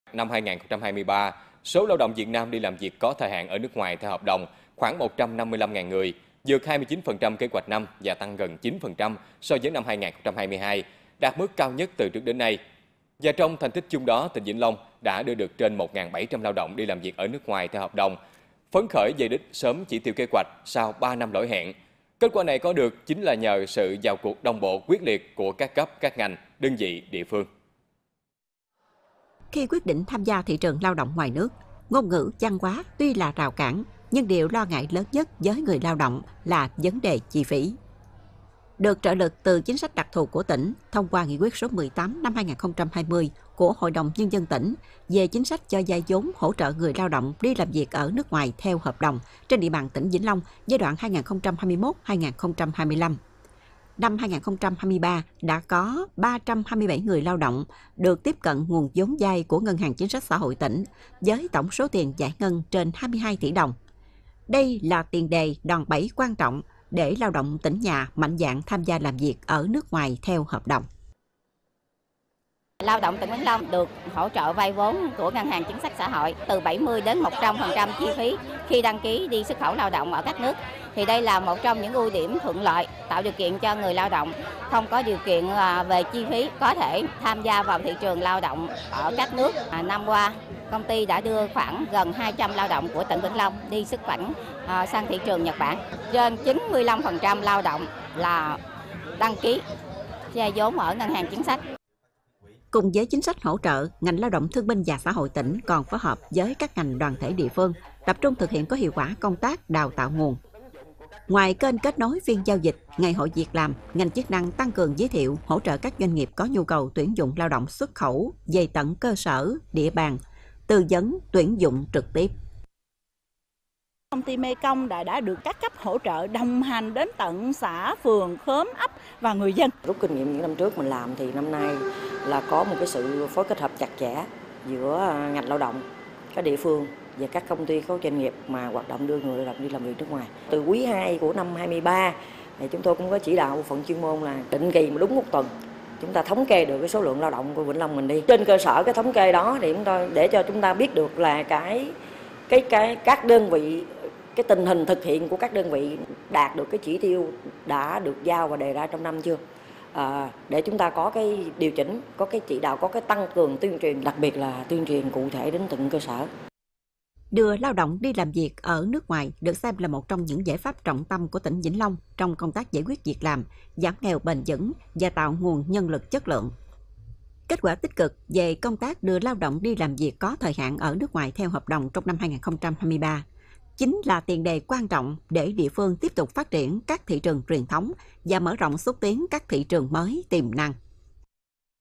Xuất khẩu lao động “về đích” sau 3 năm “lỗi hẹn”
Năm 2023, số lao động Việt Nam đi làm việc có thời hạn ở nước ngoài theo hợp đồng khoảng 155.000 người, vượt 29% kế hoạch năm và tăng gần 9% so với năm 2022, đạt mức cao nhất từ trước đến nay. Trong thành tích chung đó, tỉnh Vĩnh Long đã đưa được trên 1.700 lao động đi làm việc ở nước ngoài theo hợp đồng, phấn khởi “về đích” sớm chỉ tiêu kế hoạch sau ba năm “lỗi hẹn”. Kết quả này có được chính nhờ sự vào cuộc đồng bộ, quyết liệt của các cấp, các ngành, đơn vị, địa phương

⬇️⬇️⬇️ Tải THVL Audio để nghe đầy đủ